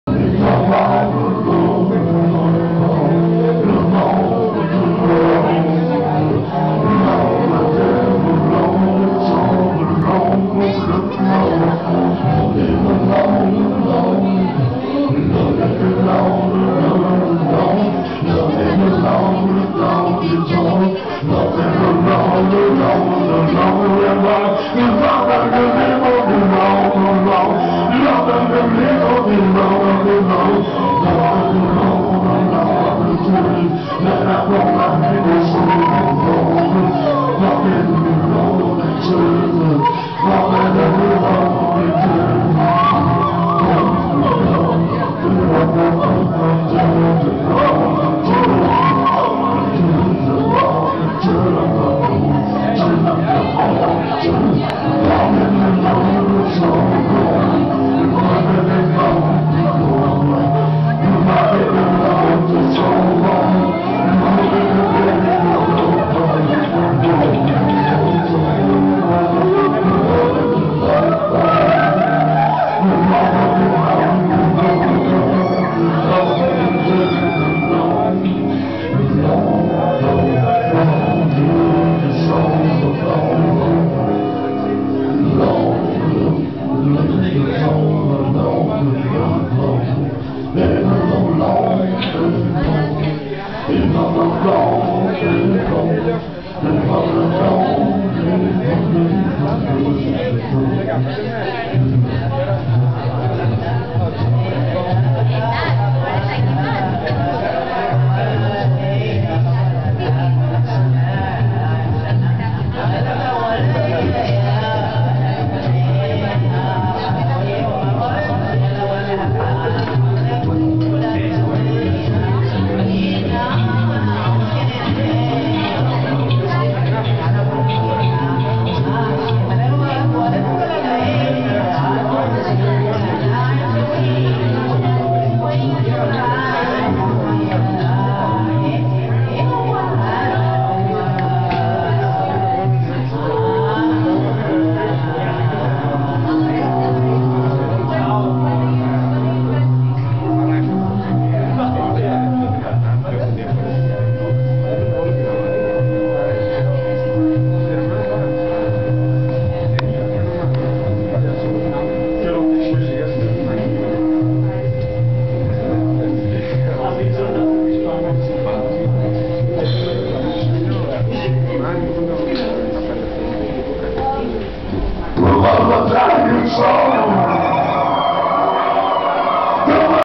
Frakture & Damo Suzuki at Magnet
amazing Frakture and Damo Suzuki gig at Magnet club in Liverpool,14 Nov 2007. I am sorry about horrible sound quality at the clip...